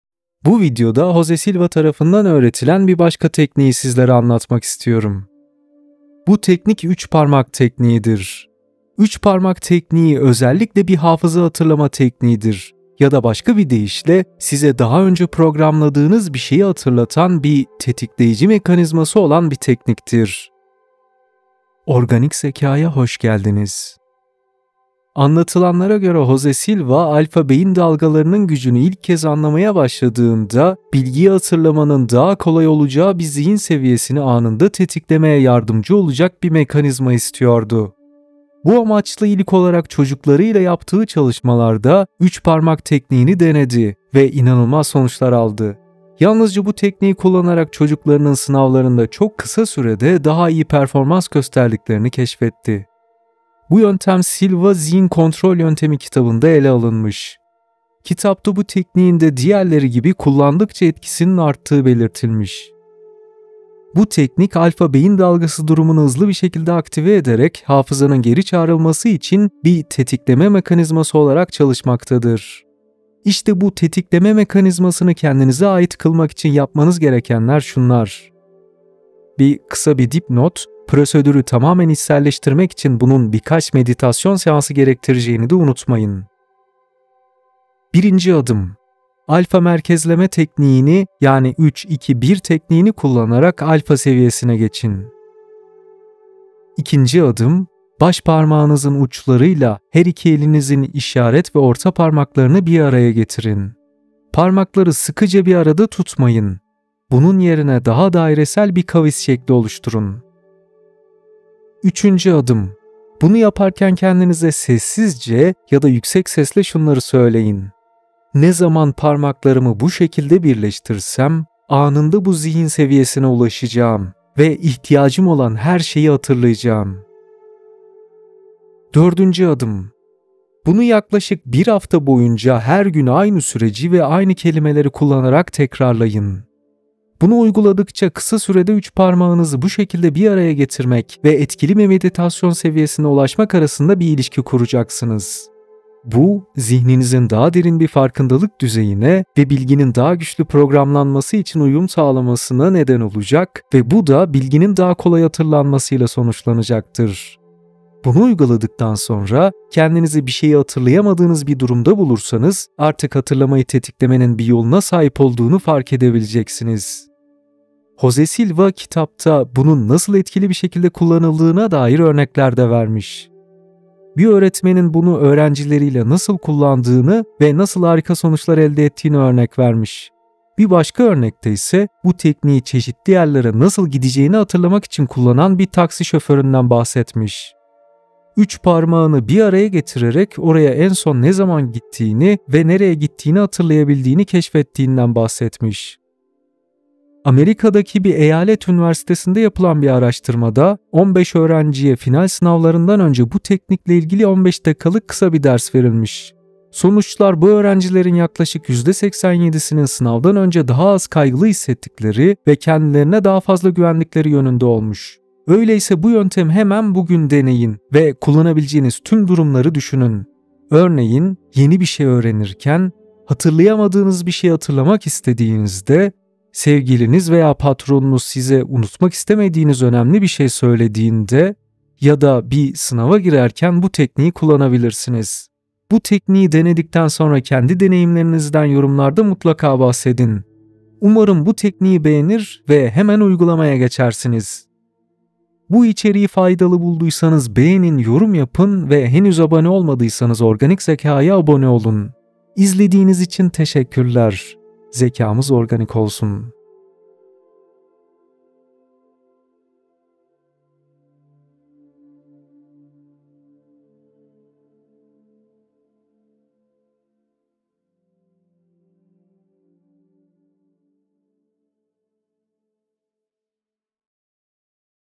ÜÇ PARMAĞINI BU ŞEKİLDE TUTUP SİHİRLİ SÖZLERİ SÖYLE | JOSE SİLVA
**Jose Silva Tekniği: Üç Parmak Tekniği**

🧠 Üç parmak tekniği, Jose Silva tarafından geliştirilen ve hafızayı hatırlatan bir tetikleyici mekanizması olan bir tekniktir.

**Tekniğin Adımları:**

1. **Silva Tekniği ile Alfa Seviyesine Geçmek **

2. **Parmak Birleştirme Hareketi Yapmak**

3. **Tetikleme Cümlesini Söylemek**

4. **Tüm adımları Tekrar Etmek **

Bu teknikle, alfa seviyesine ulaşmanız ve zihinsel farkındalığınızı derinleştirmeniz arasında bir bağlantı kuracaksınız. Bu, bilginin daha etkili bir şekilde programlanmasına ve hatırlanmasına olanak tanır.

**Sonuçlar ve Uygulamalar:**

Bu teknik, öğrencilerin sınav kaygısını azaltmalarına ve kendilerine güvenlerini artırmalarına yardımcı olabilir. Ayrıca, hatırlanması gereken bilgileri hatırlamak için kullanılabilir, yeni öğrenilen konuları pekiştirmek için idealdir.

Deneyimlerinizi paylaşarak bu tekniğin etkisini keşfetmek için hemen uygulamaya geçin! 🌟

Zihinsel yeteneklerinizi geliştirmek için bir sonraki videoda görüşmek üzere.

Zekamız Organik olsun!⚡️🧠